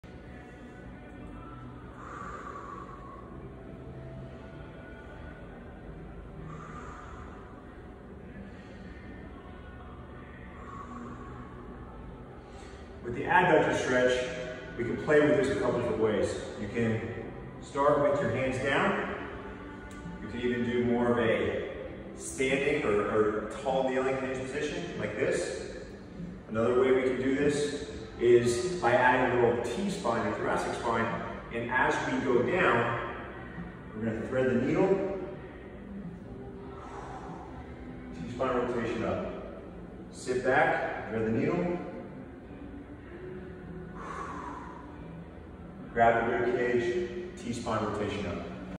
Mobility - Adductor Rock Backs / Thread the needle option
Here we are playing with different positions. You are listening to your body and seeking positions that feel best to you.

One option I throw in toward the end is the thread the needle option. Great for rotational athletes and weekend warriors!